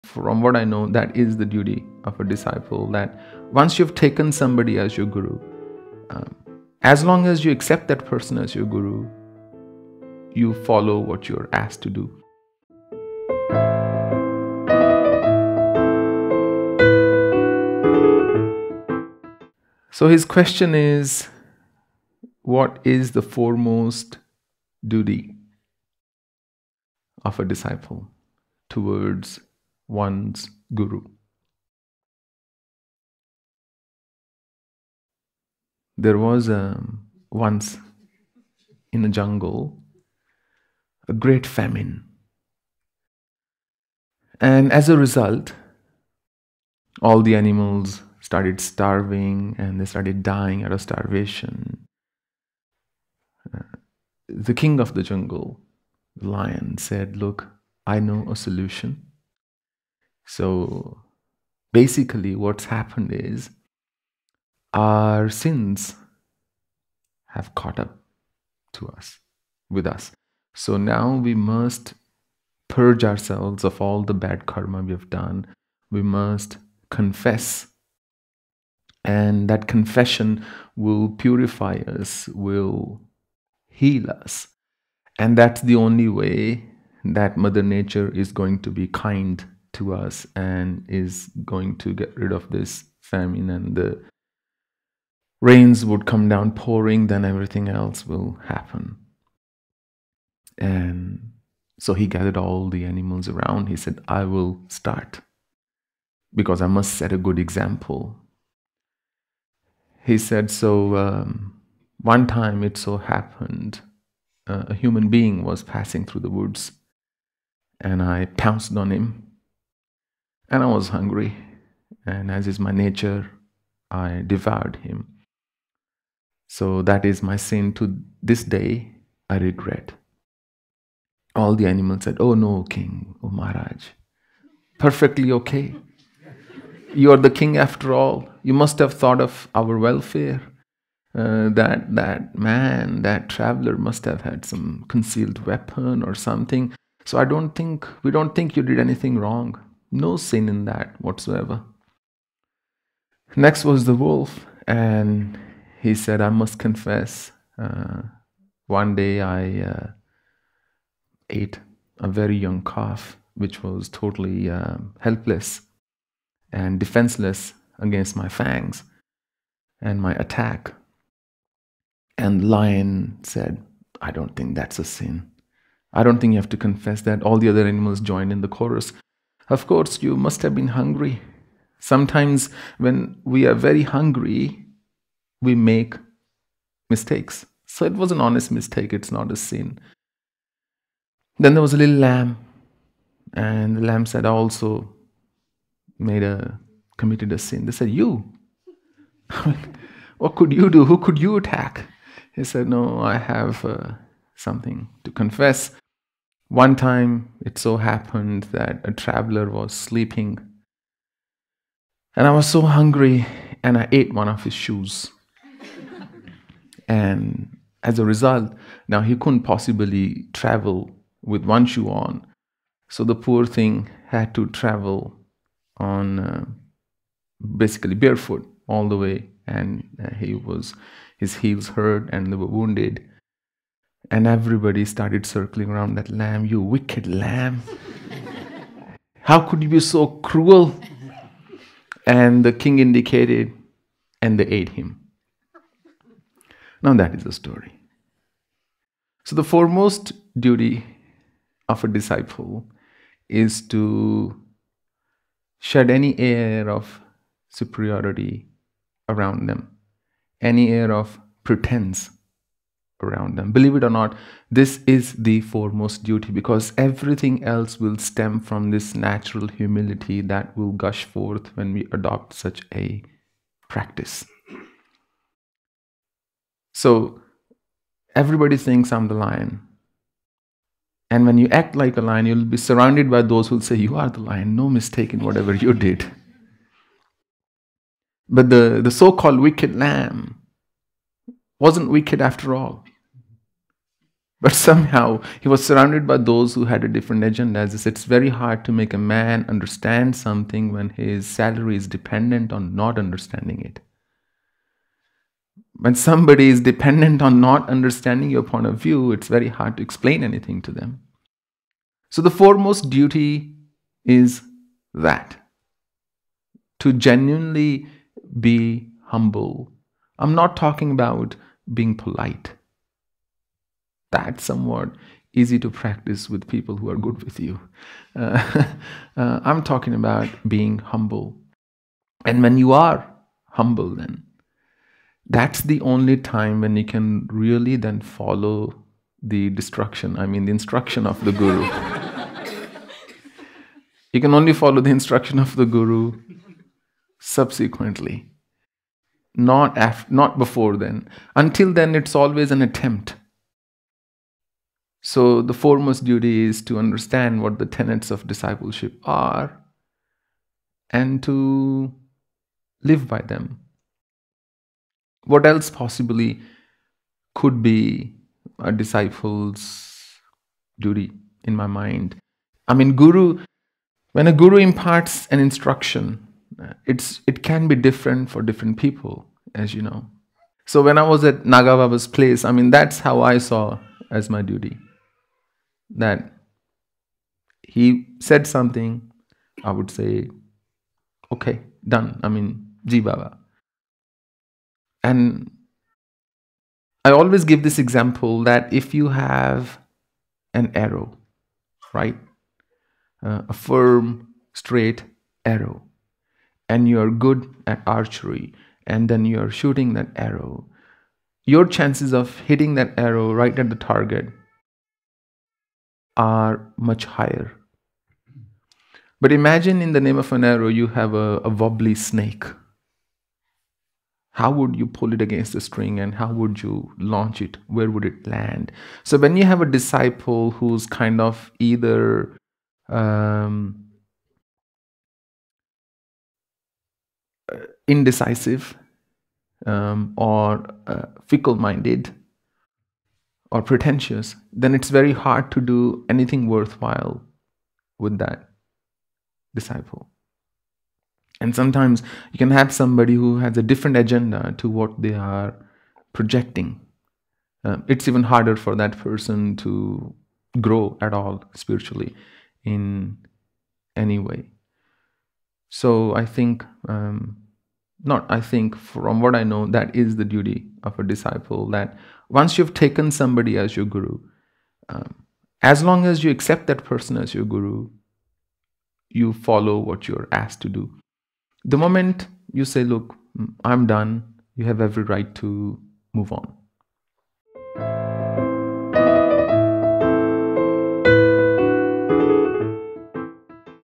The Story of the 'Wicked' Lamb: A Lesson for Disciples
On the sacred occasion of Guru Purnima, Om Swami elucidates the foremost duty of a disciple with a powerful story about an arrogant lion and its cunning followers.

This discourse is part of the Guru Purnima special series. It was originally recorded at the Sri Badrika Ashram in March 2021.